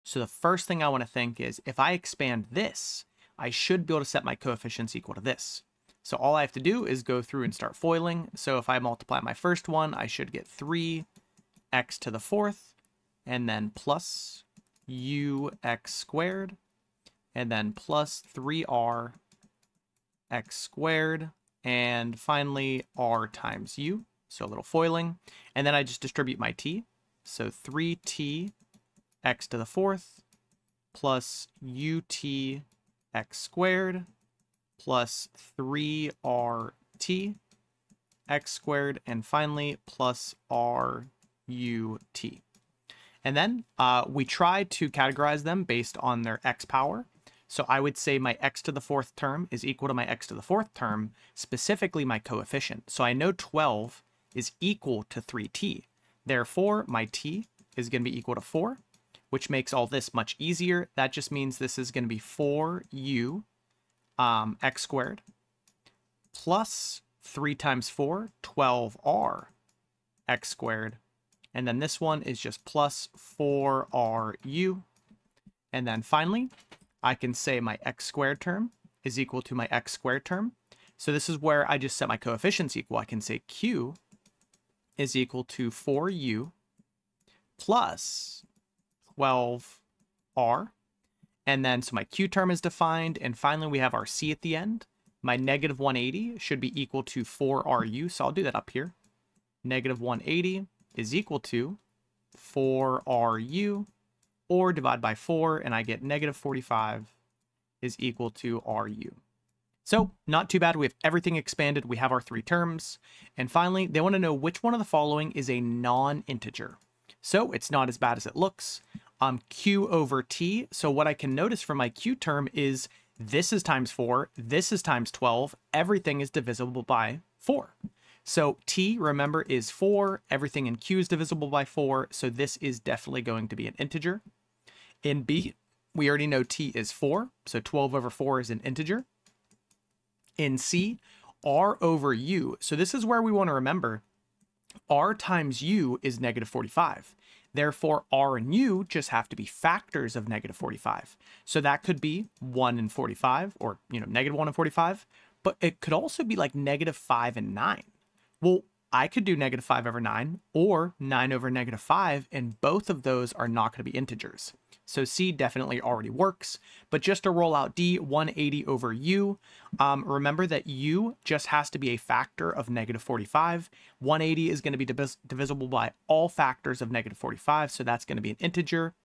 *real* October 2025 SAT: Expanding quadratics
*NOT* from Bluebook — these questions were *mentioned heavily* in the October SAT Reddit threads and mirrors the real test’s difficulty and tone.

📘 Question Transcript:
- expanding quadratics
- quartics factoring foiling